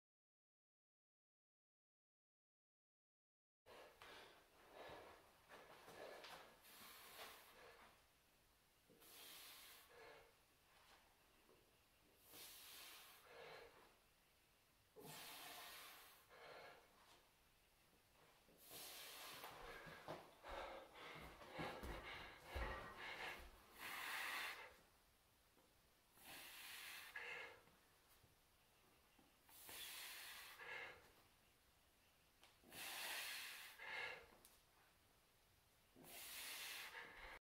High Plank Shoulder Taps
This is a shoulder and scapular stability exercise as well as an anti-extension and rotational core strength challenge. Keep your feet wider to make it easier. Do not let your shoulders or hips rotate and move as slowly as possible while tapping your shoulders. Do not sag between your shoulder blades and maintain a hollowed out core position.